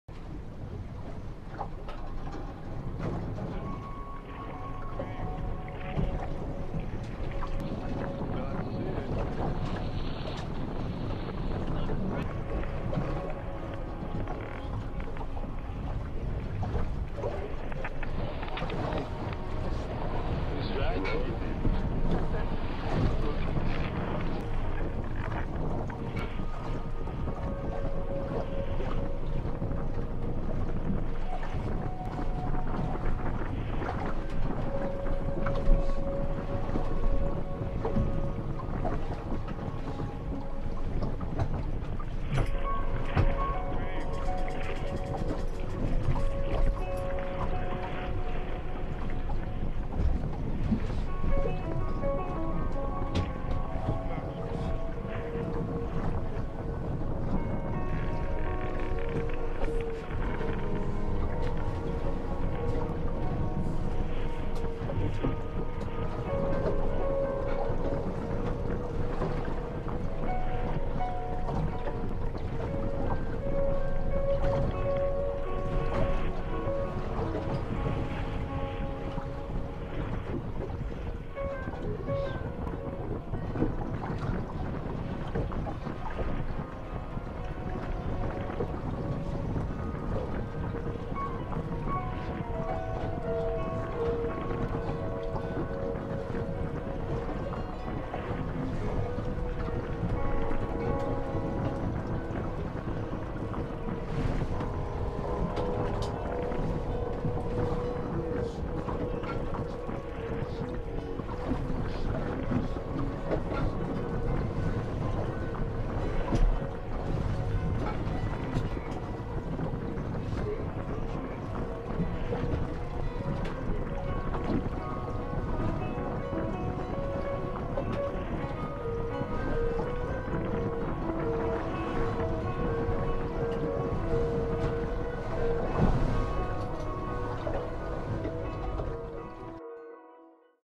Beautiful Sail
Del Corazon a Cheoy Lee offshore 31 ketch sails herself through San Francisco bay time warp.